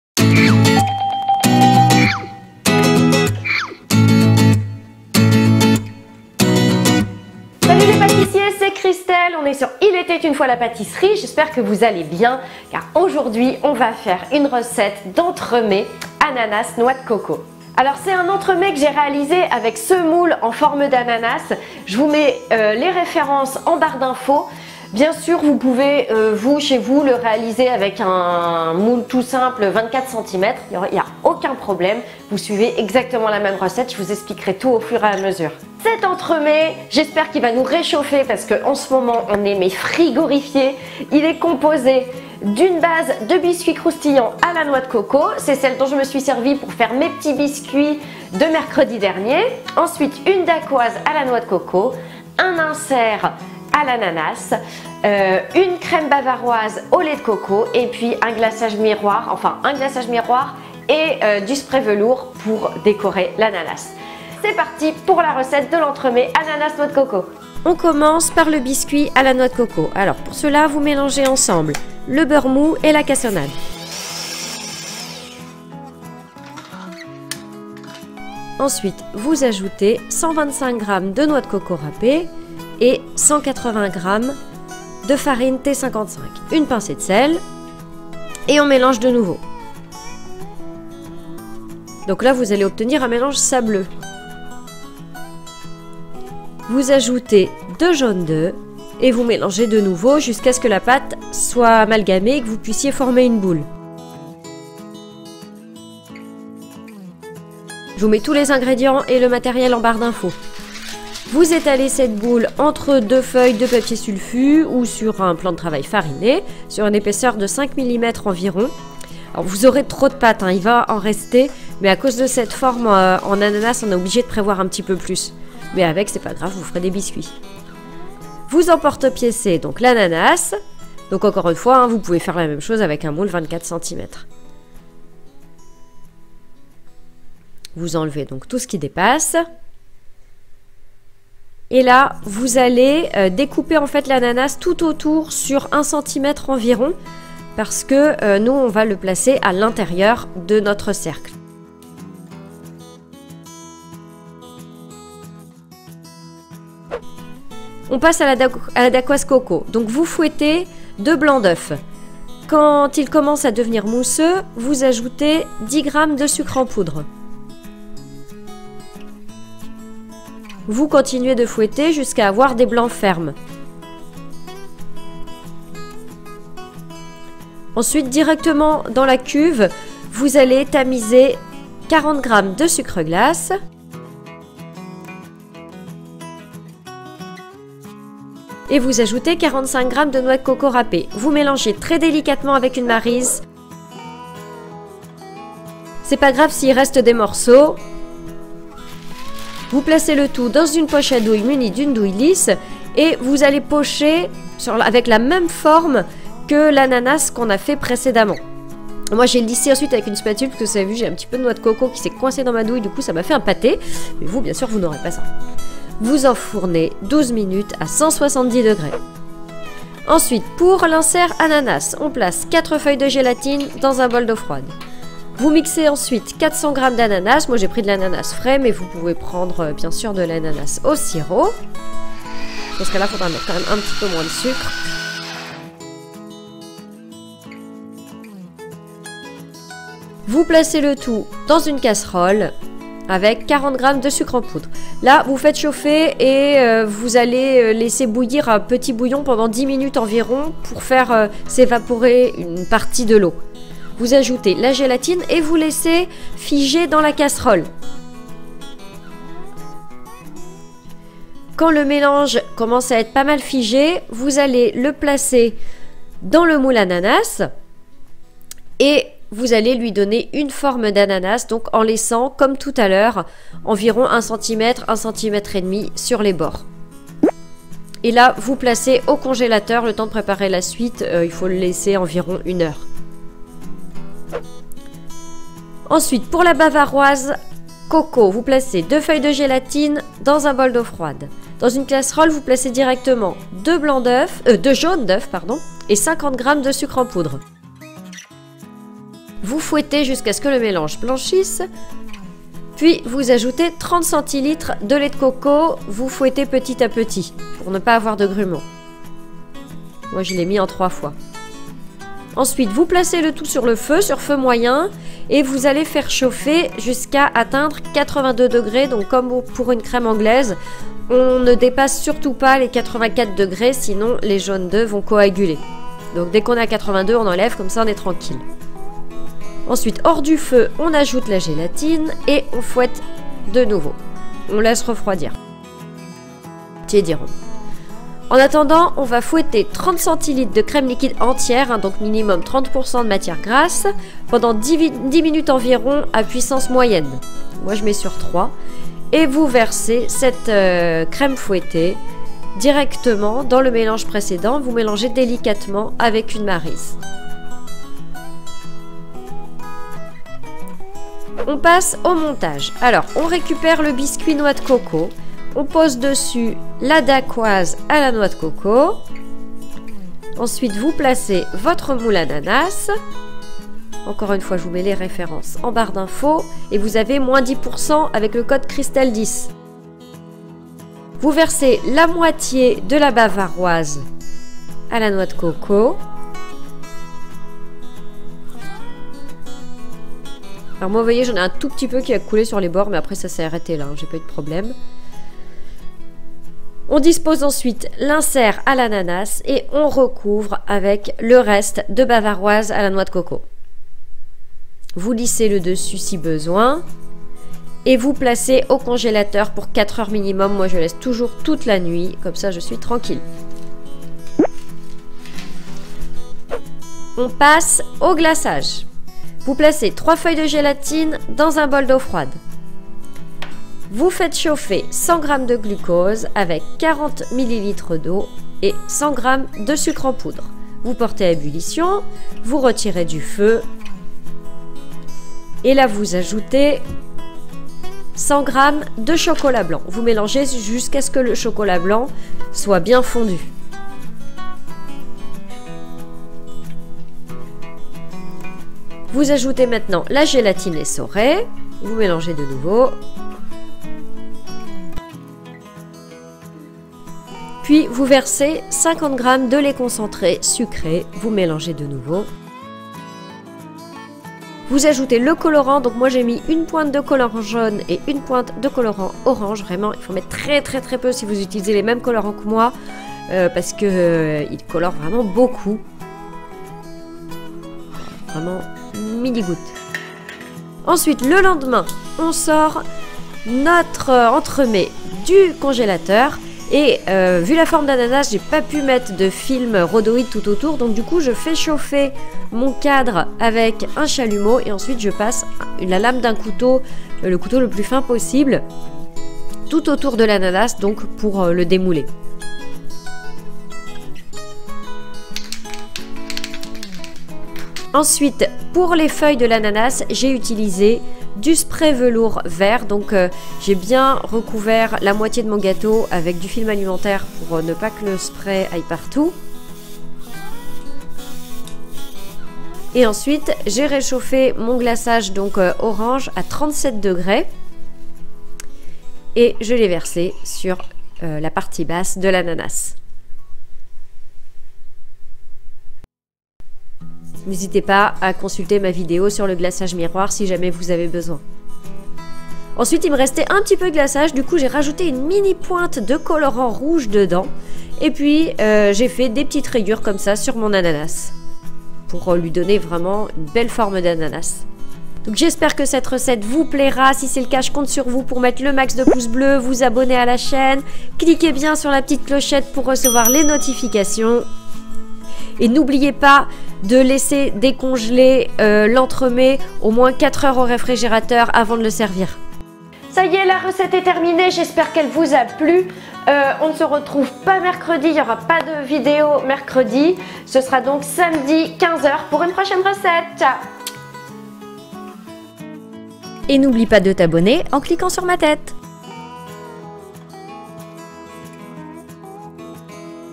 🍍 ENTREMETS ANANAS NOIX DE COCO ~ Gâteau en forme d'ananas ! 🍍
Un délicieux entremets ananas noix de coco en forme d’ananas ! ❤ Déroule la barre d'info pour les ingrédients et le matériel ❤

Matériel nécessaire à la recette de l’entremets ananas noix de coco :
🍍 Moule ananas -10% avec le code CHRISTELLE10
🍍 Colorants jaune, orange et rouge (idem)
🍍 Plaques de cuisson
🍍 Thermomètre de cuisine
🍍 Rouleau à pâtisserie anti-adhésif
🍍 Poche à douille
🍍 Douille lisse 10 mm

Ingrédients pour un entremets ananas noix de coco de 8 personnes :
Biscuits croustillant à la noix de coco :
• 160 g de beurre mou
• 120 g de sucre roux
• 125 g de noix de coco râpée
• 180 g de farine
• 1 pincée de fleur de sel
• 2 jaunes d’œuf
Dacquoise noix de coco :
• 2 blancs d'oeuf
• 10 g de sucre en poudre
• 40 g de sucre glace
• 45 g de noix de coco en poudre
 Pour l'insert ananas :
• 400 g d'ananas
• 40 g de sucre en poudre
• 4 feuilles de gélatine
 Mousse bavaroise à la vanille :
• 30 cl de lait de coco
• 2 jaunes d'oeuf
• 50 g de sucre en poudre
• 2 feuilles de gélatine
• 30 cl de crème liquide entière
 Glaçage :
• 3 feuilles de gélatine
• 40 ml d'eau
• 100 g de sucre
• 100 g de glucose -10% avec le code CHRISTELLE10)
• 100 g de chocolat blanc
• 50 g de lait concentré sucré
• Colorants jaune, rouge et orange